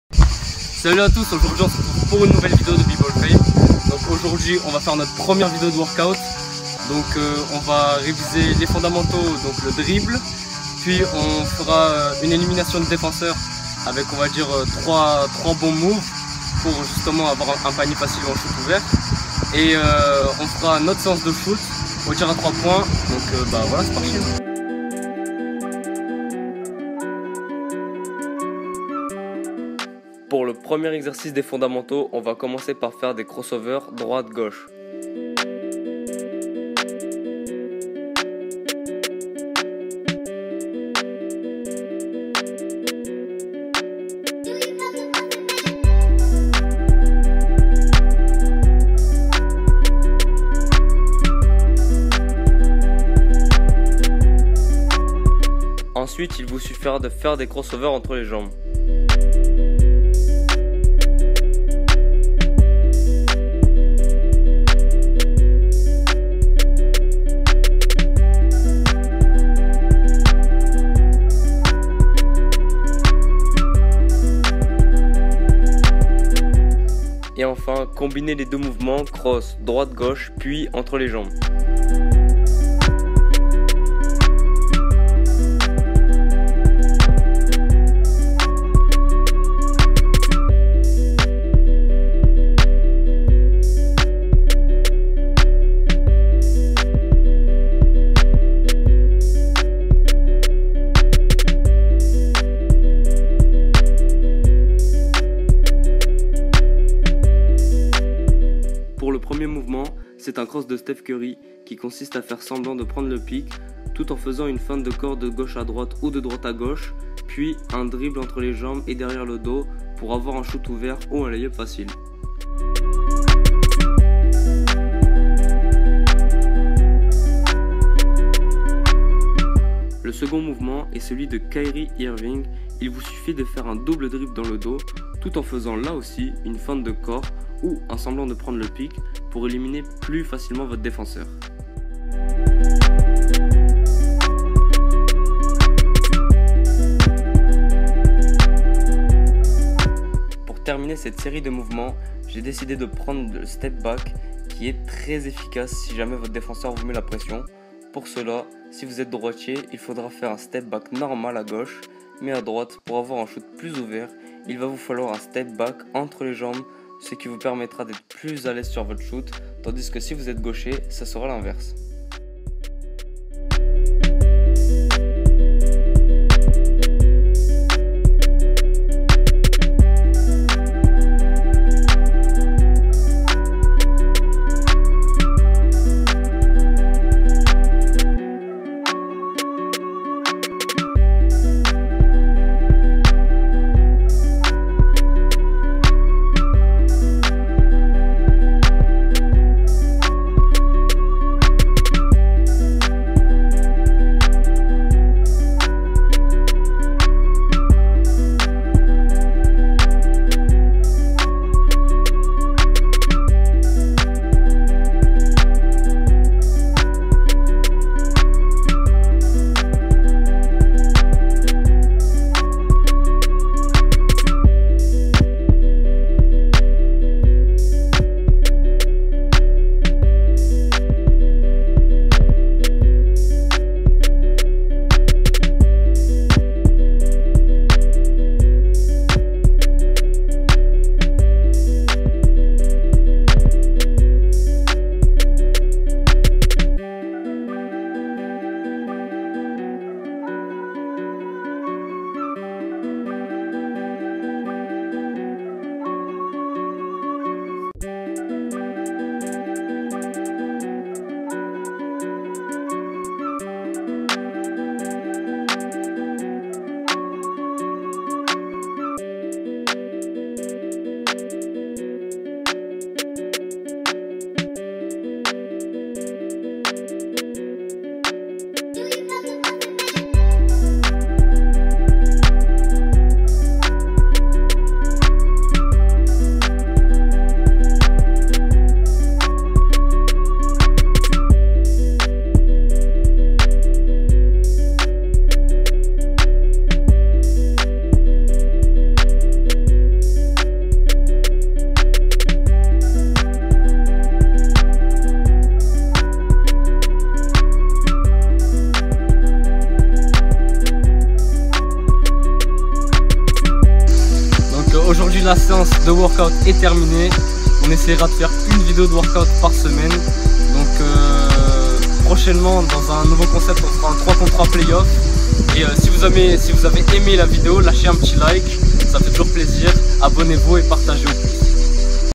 WORKOUT #1 (dribbles, shoot)
Premiere video d'une longue serie de workout, on essayera de publier au moins une video par semaine de ce type.
Désolé pour le bruit de cigale en fond, on essayera de corriger ça